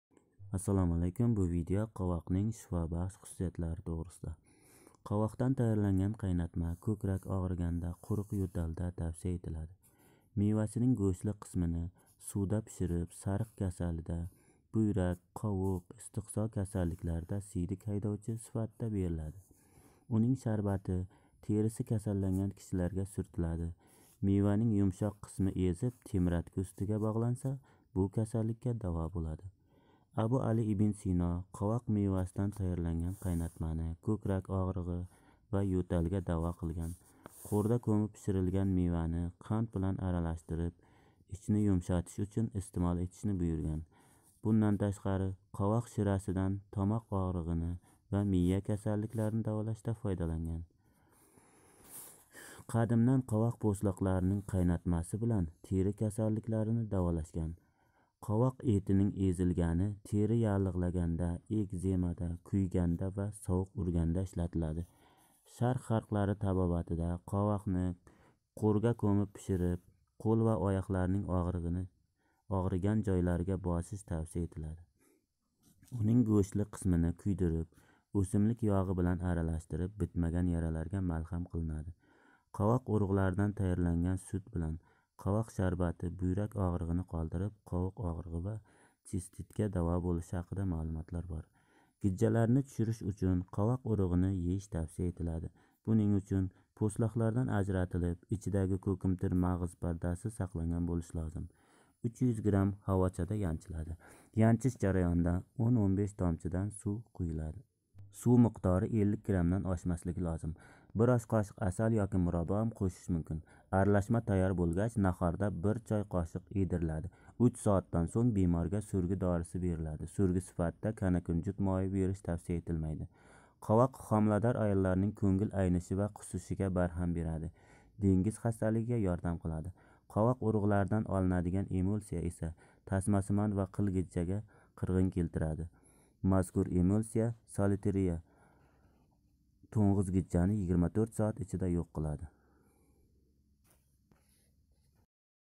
Хомладор аёллар кўнгил айнишига малҳам, Гижжага қарши кучли таббий восита - Қовоқ
гижжа давоси